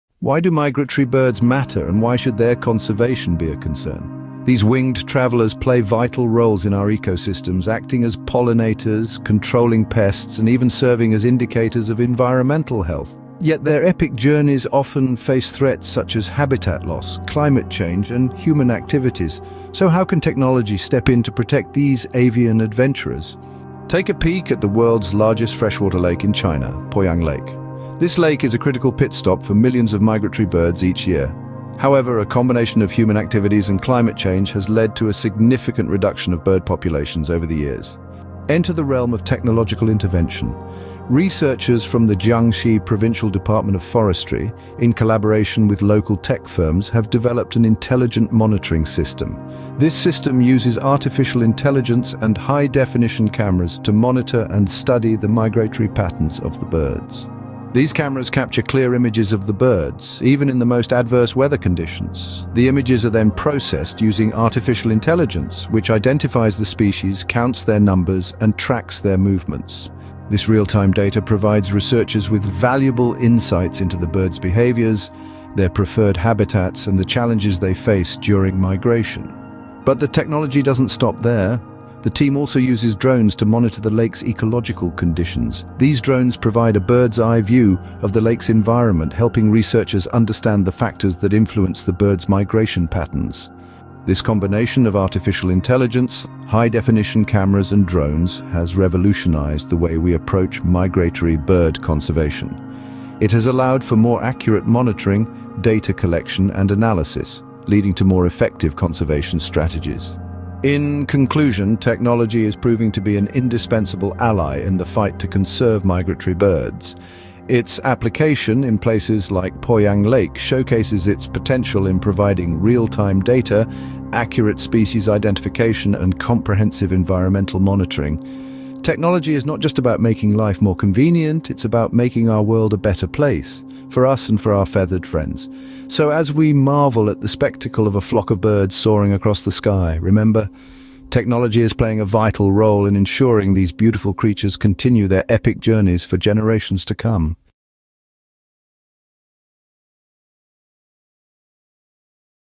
Tech Wings Bird Conservation at China's
Does China have robot birds?
The idea might seem far fetched, but robotic birds are very much a reality, and China has been using them to surveil people across the country.